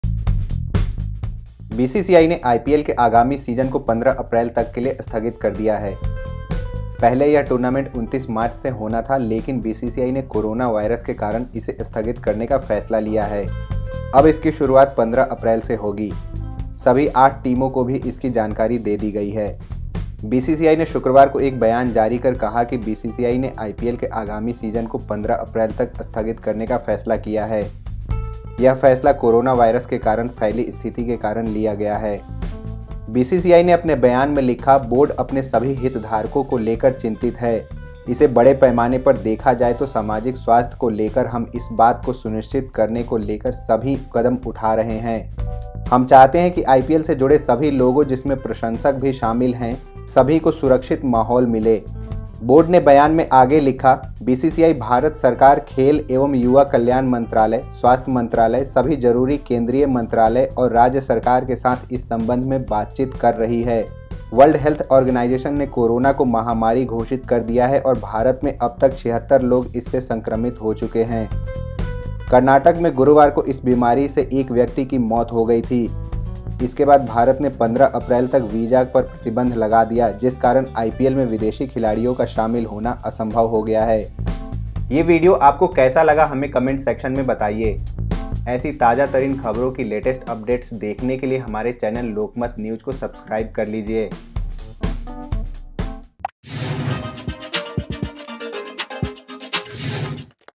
Corona Virus की वजह से 15 April तक टला IPL
बीसीसीआई ने आईपीएल के आगमी सीजन को 15 अप्रैल तक के लिए स्थागित कर दिया है। पहले यह टूर्नामेंट 29 मार्च से होना था, लेकिन बीसीसीआई ने कोरोनो वायरस के कारण इसे स्थागित करने का फैसला लिया है और अब इसकी शुरुआत 15 अप्रैल से होगी। सभी 8 टीमों को भी इसकी जानकारी दे दी गई है।